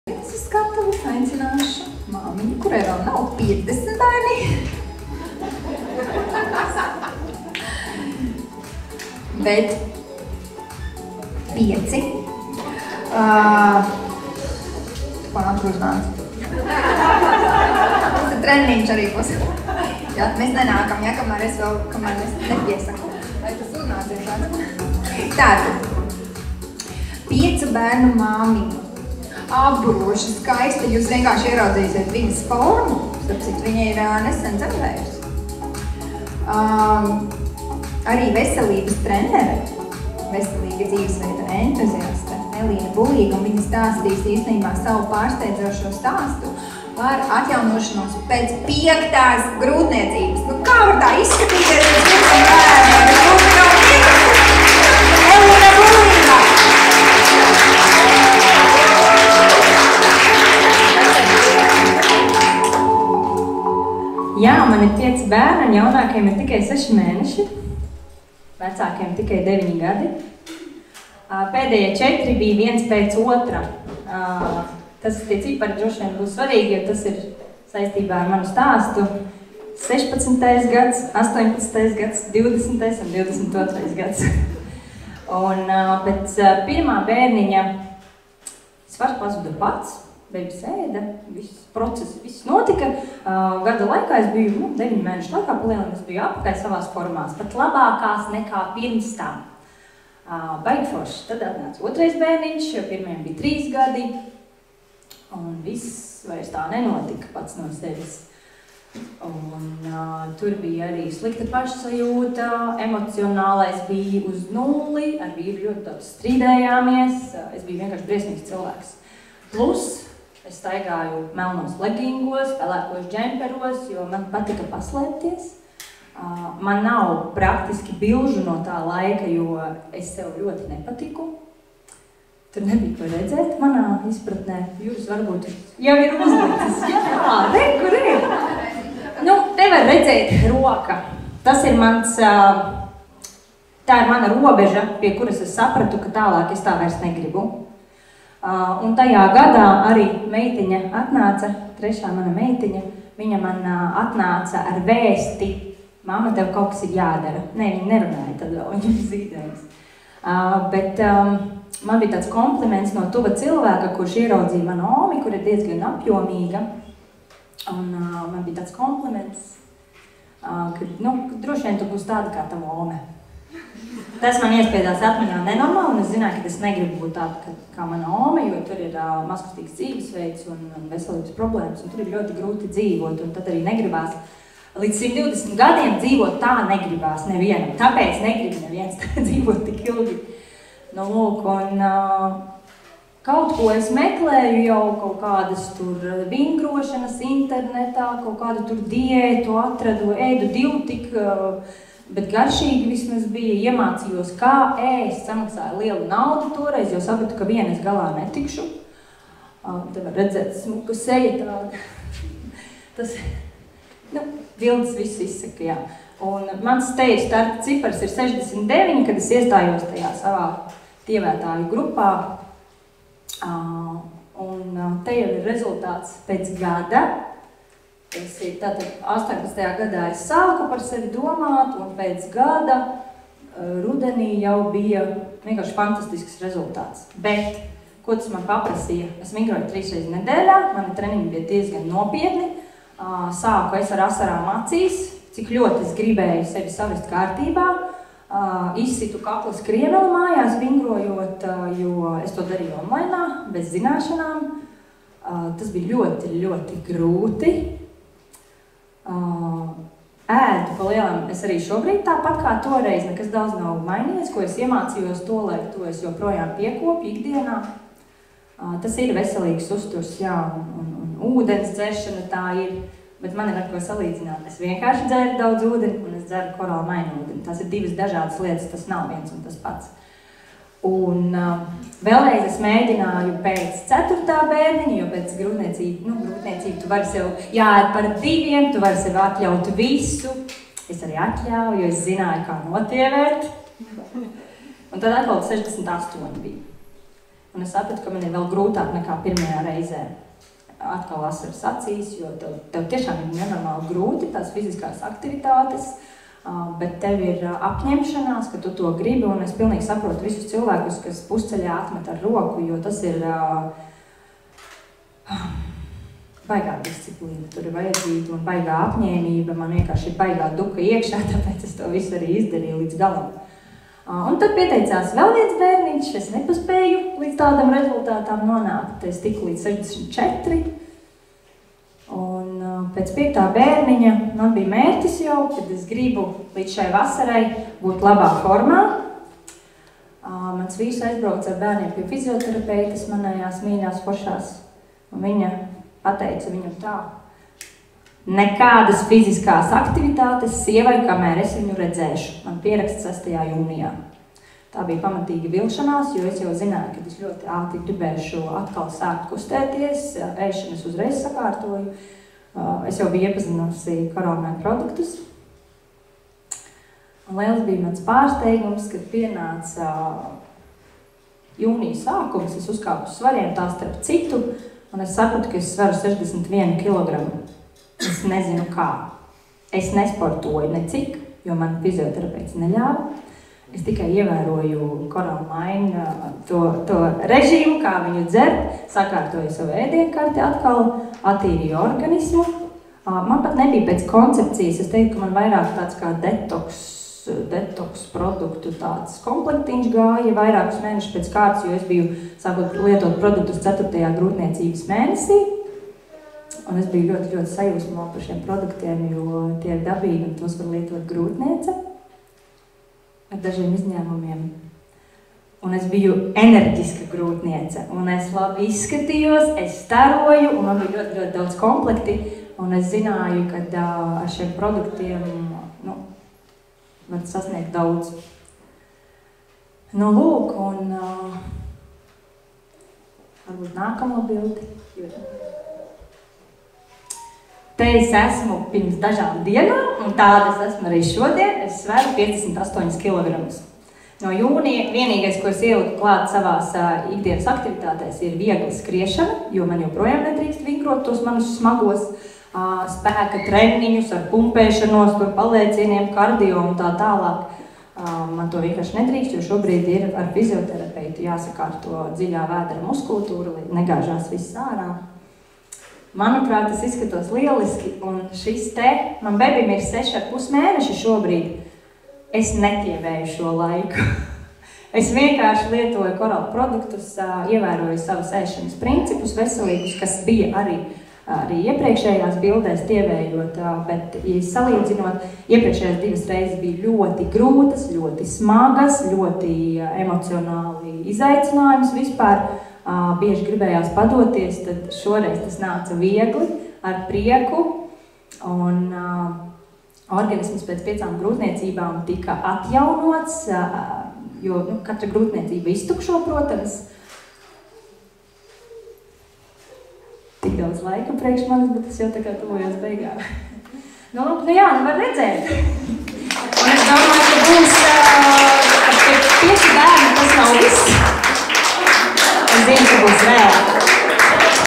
Elīna Bulīga: Pārsteidzošā ATJAUNOŠANĀS pēc piektās GRŪTNIECĪBAS jeb nutrientu spēks
Coral Stories 2022

Elīna Bulīga – 5 bērnu māmiņa, veselīga dzīvesveida entuziaste. “Pārsteidzošā ATJAUNOŠANĀS pēc piektās GRŪTNIECĪBAS jeb nutrientu spēks”